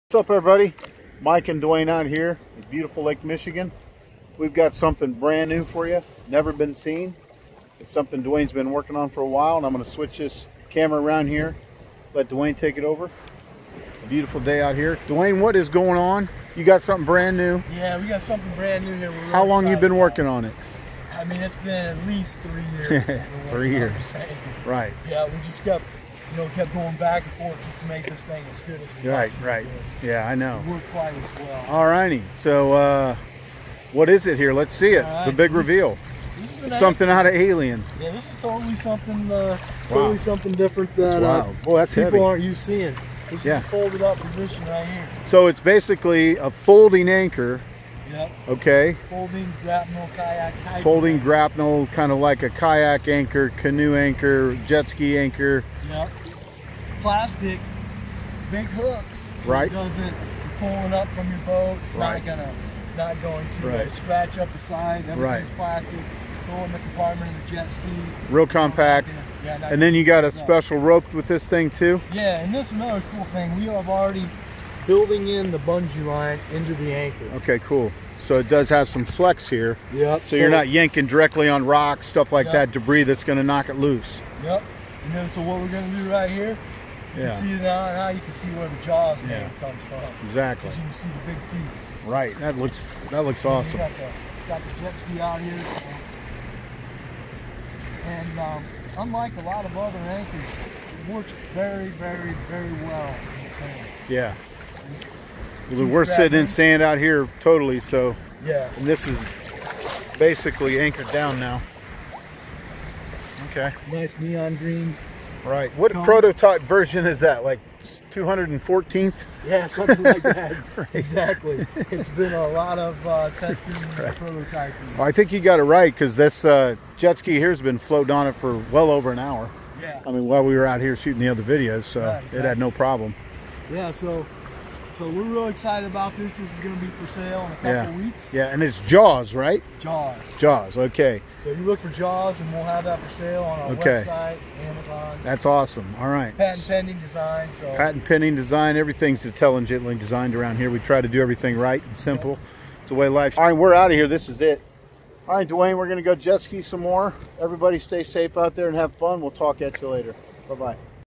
JAWS Grapnel Style Anchor review & demo on Lake Michigan by SandShark Anchor
JAWS Anchor by SandShark, New! Grapnel Type Folding Anchor Kit: 30 ft Bungee Rope-Line System, Easy Storage, No Boat Damage. Built for: Jetski, Canoe, PWC, Kayak, Paddleboard, Inflatable, Small Boat.